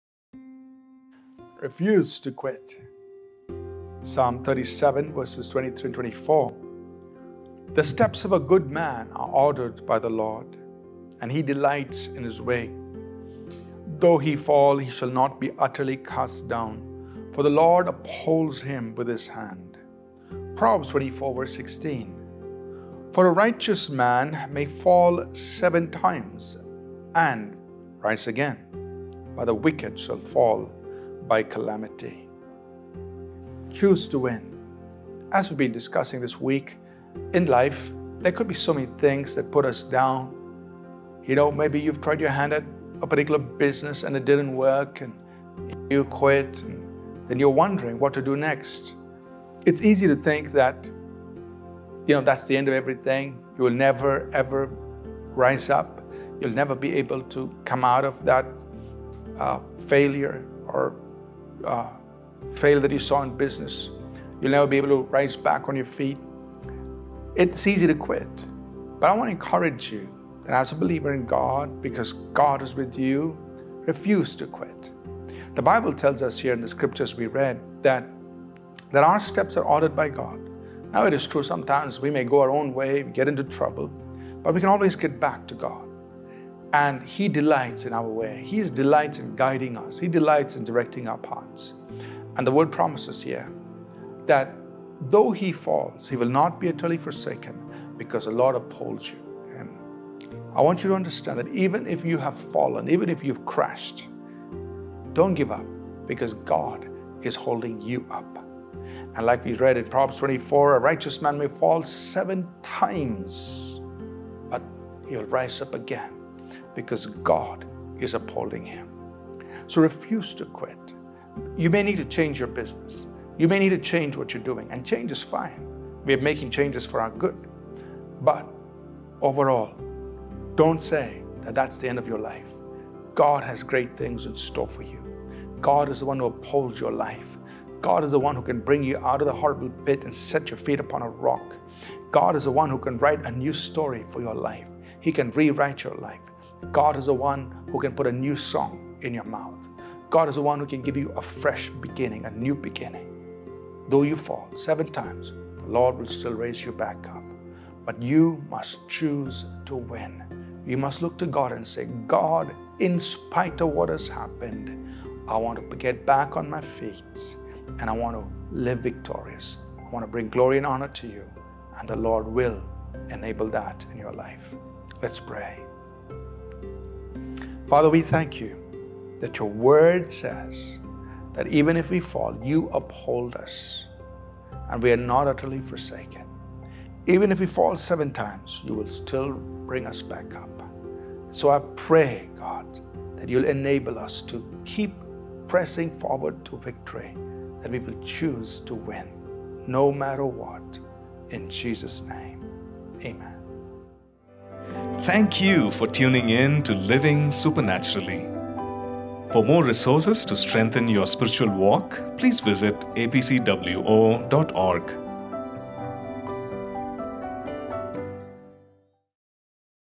Refuse to quit - Ashish Raichur | Daily Devotion, July 4
Today's Devotional - Refuse to quit

Psalm 37:23-24
23 The steps of a good man are ordered by the LORD, And He delights in his way.
24 Though he fall, he shall not be utterly cast down; For the LORD upholds him with His hand.

Proverbs 24:16
For a righteous man may fall seven times And rise again, But the wicked shall fall by calamity.